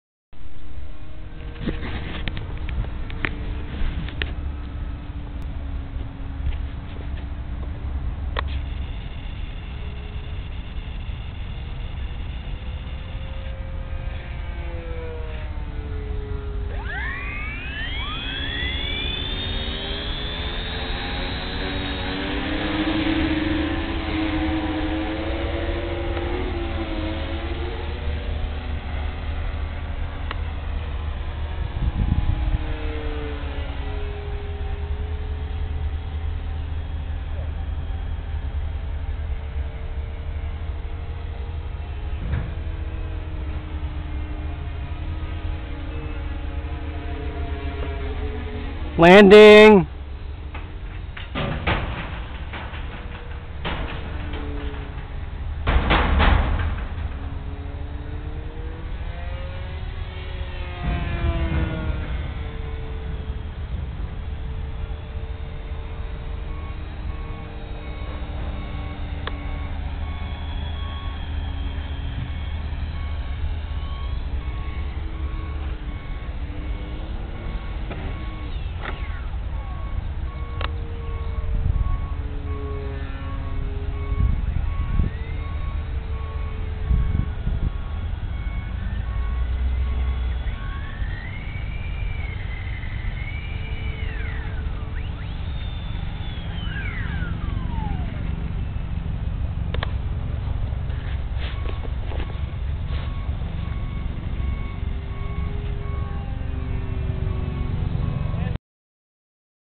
VTOL -ThirdFullConversion.AVI
Third full VTOL conversion. Hover takeoff to 90 degree flyby to hover landing.